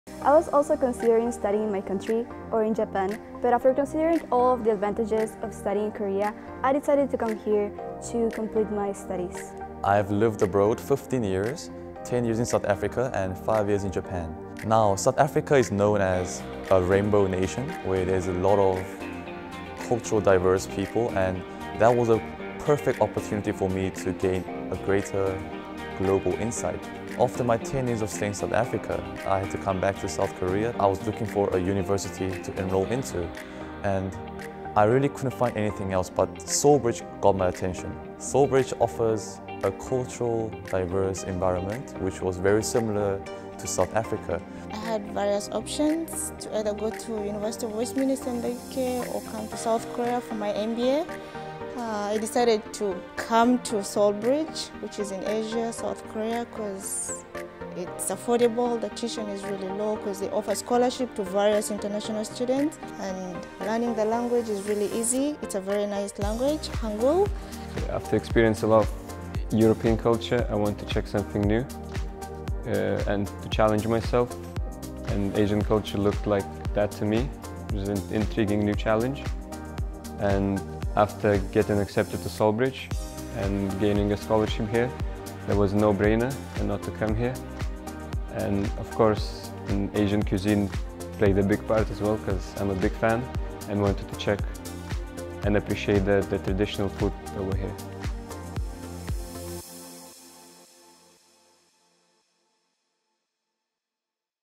Why I chose to study in South Korea | SolBridge International School of Business
SolBridge students share with us why they chose South Korea as their study abroad destination.